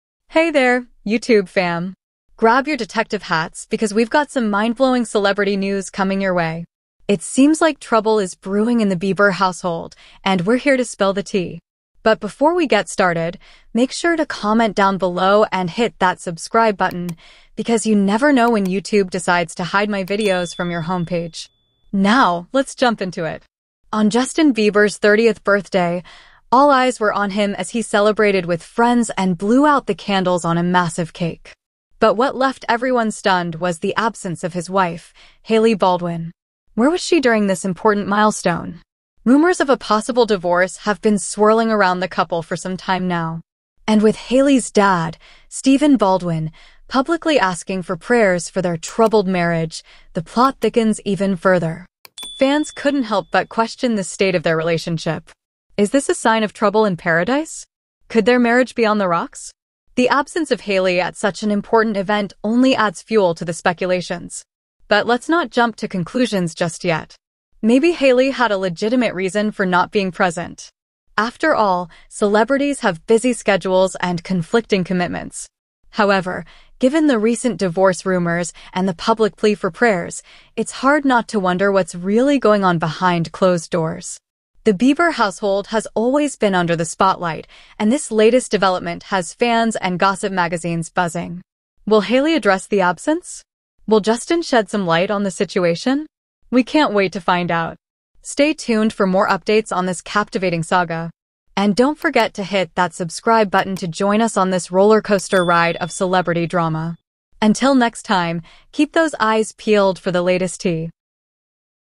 Hailey Baldwin was UNAPOLOGETICALLY absent at Justin Bieber's birthday party ......
selenagomez selenagomeznews hollywood jelena justinbieberbirthday2024 justinbeiber haileybieber Hey there, YouTube fam! Grab your detective hats because we've got some mind-blowing celebrity news coming your way. It seems like trouble is brewing in the Bieber household, and we're here to spill the tea.

On Justin Bieber's 30th birthday, all eyes were on him as he celebrated with friends and blew out the candles on a massive cake. But what left everyone stunned was the absence of his wife, Hailey Baldwin. Where was she during this important milestone?

Rumors of a possible divorce have been swirling around the couple for some time now. And with Hailey's dad, Stephen Baldwin, publicly asking for prayers for their troubled marriage, the plot thickens even further.

Fans couldn't help but question the state of their relationship. Is this a sign of trouble in paradise? Could their marriage be on the rocks? The absence of Hailey at such an important event only adds fuel to the speculations.

But let's not jump to conclusions just yet. Maybe Hailey had a legitimate reason for not being present. After all, celebrities have busy schedules and conflicting commitments. However, given the recent divorce rumors and the public plea for prayers, it's hard not to wonder what's really going on behind closed doors.

The Bieber household has always been under the spotlight, and this latest development has fans and gossip magazines buzzing. Will Hailey address the absence? Will Justin shed some light on the situation? We can't wait to find out.

Until next time, keep those eyes peeled for the latest tea!